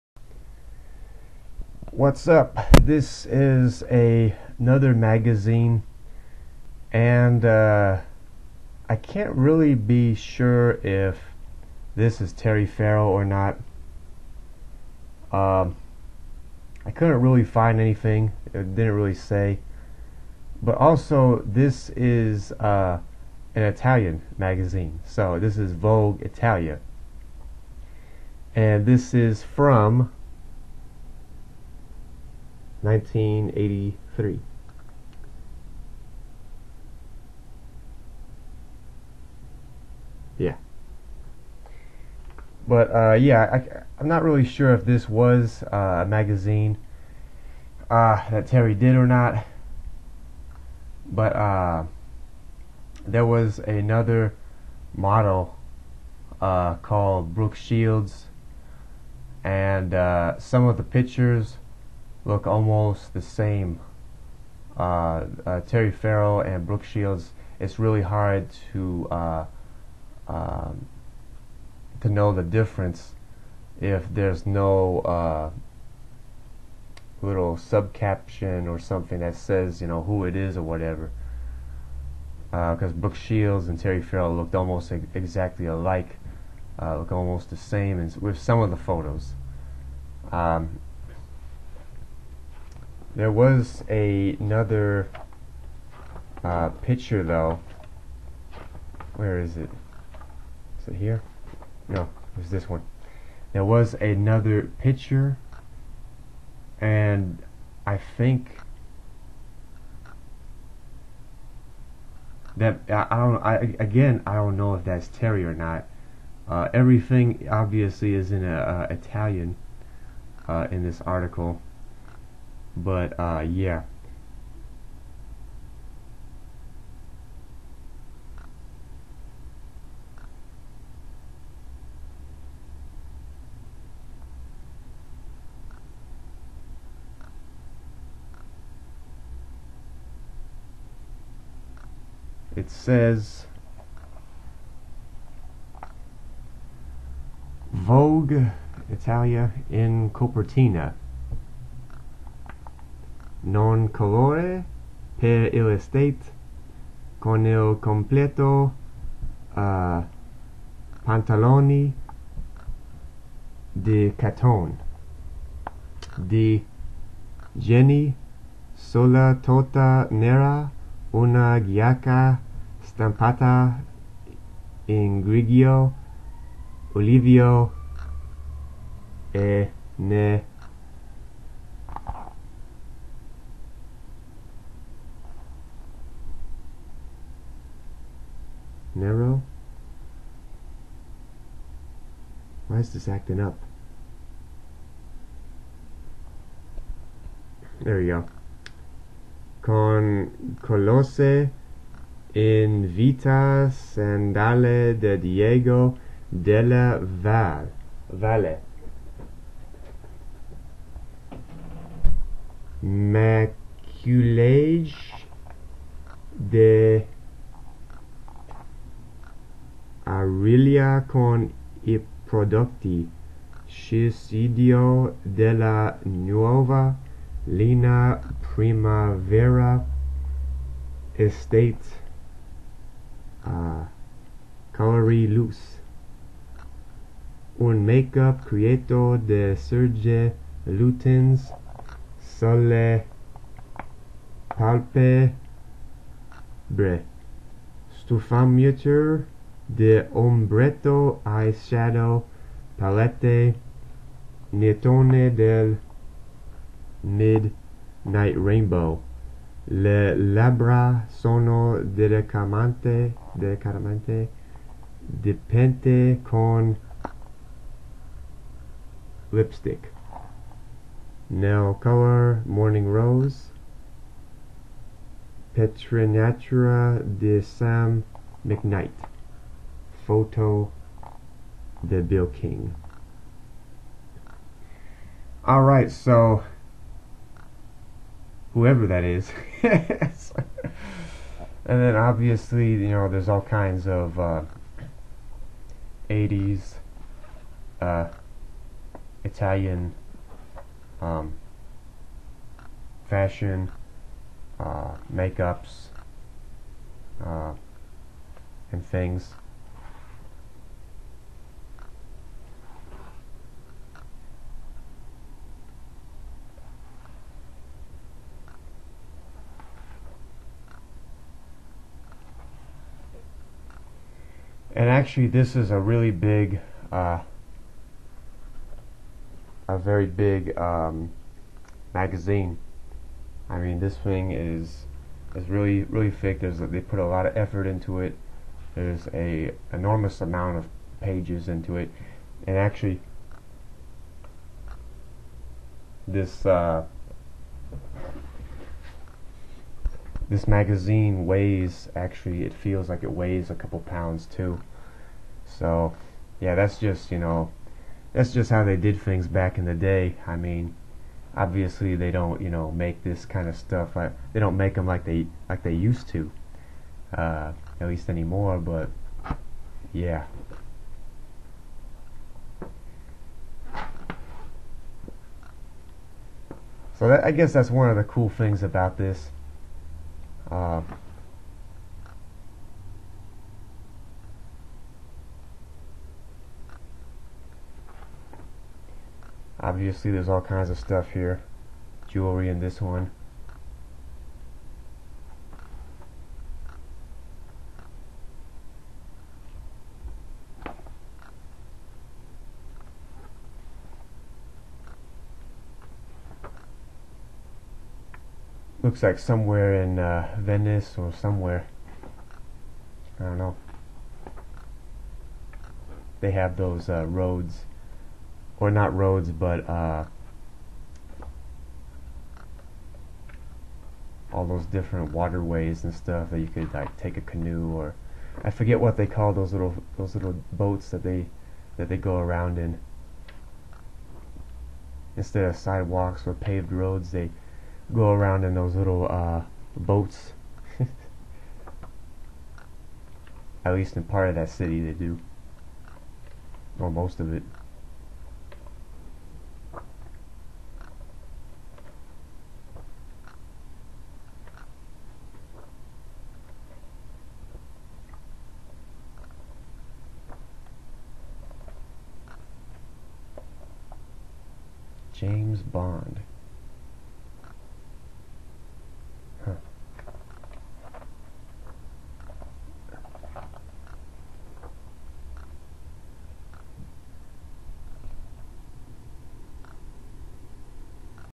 Terry Farrell Possible 1983 Vogue Italia Magazine
I don't know if this is Terry. But as a kid I collected random stuff like this. It could be Terrys look alike Brooke Shields. Since some of their photos are very simular and tough to tell apart. This magazine was in Italian and didn't say who the cover picture was